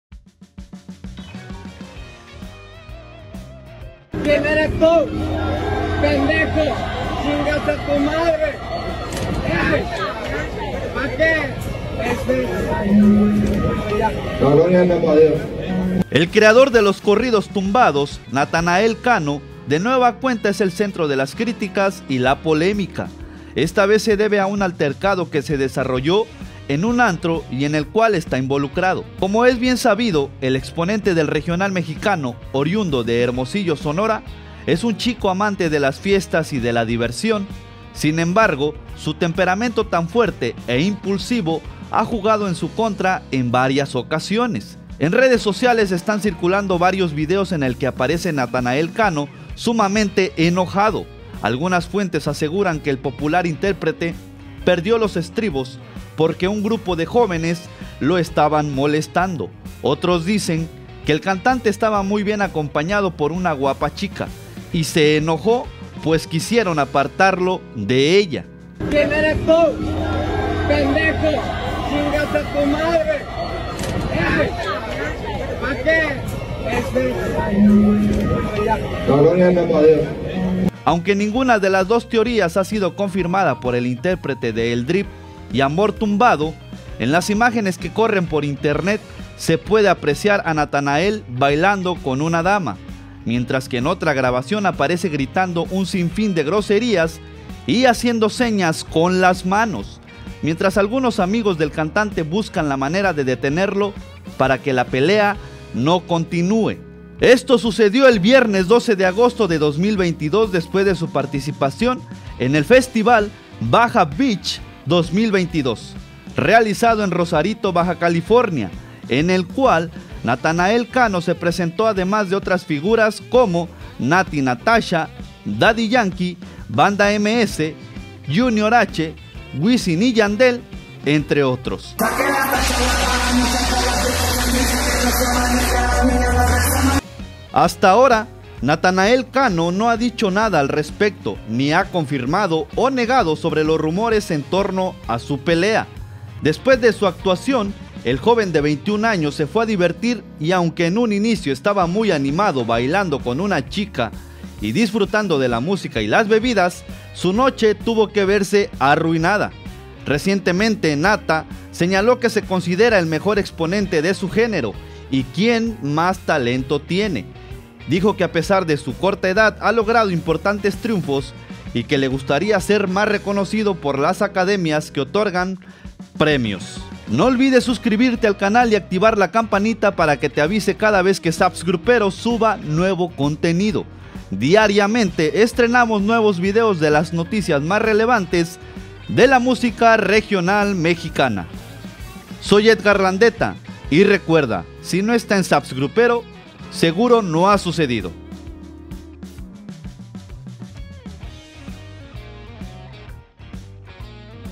Natanael Cano inicia riña en un antro de Rosarito
En redes sociales están circulando varios vídeos en el que aparece Natanael Cano sumamente enojado, algunas fuentes aseguran que el popular intérprete perdió los estribos porque un grupo de jóvenes lo estaban molestando, otros dicen que el cantante estaba muy bien acompañado por una guapa chica y se enojó, pues quisieron apartarlo de ella.

Y recuerda...
¡Si no está en SAPS Grupero... seguro no ha sucedido!